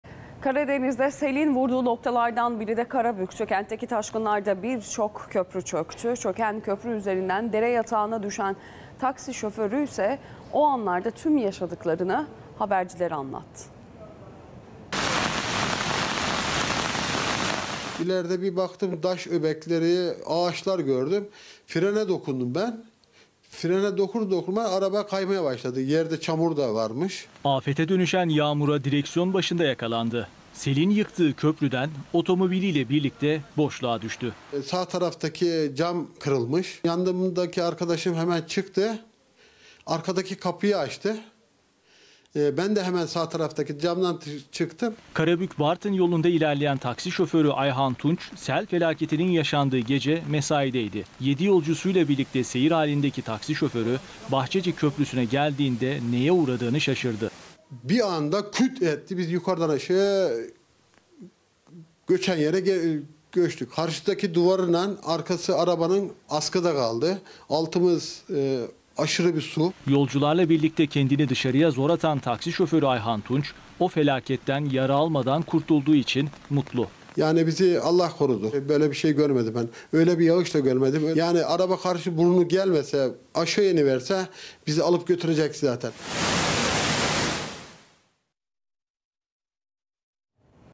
Taksi şoförü aracıyla çöken köprüden uçtu
Karadeniz'de selin vurduğu noktalardan biri de Karabük'tü. Kentteki taşkınlarda bir köprü çöktü. Çöken köprü üzerinden dere yatağına düşen taksi şoförü, anlarda yaşadıklarını habercilere anlattı.